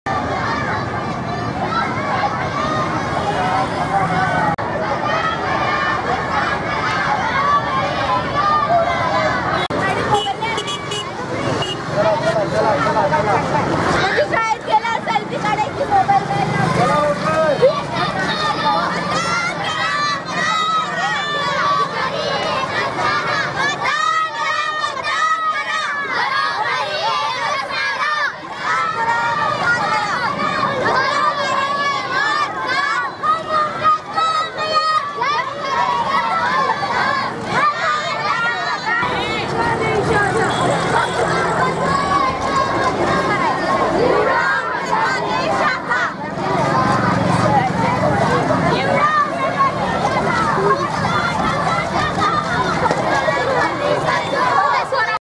धुळे महापालिका निवडणूक रणधुमाळीः मतदान जनजागृतीसाठी विद्यार्थिनींची रॅली
धुळे - महापालिका निवडणुकीच्या पार्श्वभूमीवर शहरातील कमलाबाई कन्या शाळेच्या विद्यार्थिनींनी रॅली काढून मतदान करण्याबाबत आवाहन केले आहे. या रॅलीतून विद्यार्थिनींनी मतदानाबाबत जनजागृती केली. या रॅलीने धुळेकर नागरिकांचे लक्ष वेधून घेतले आहे.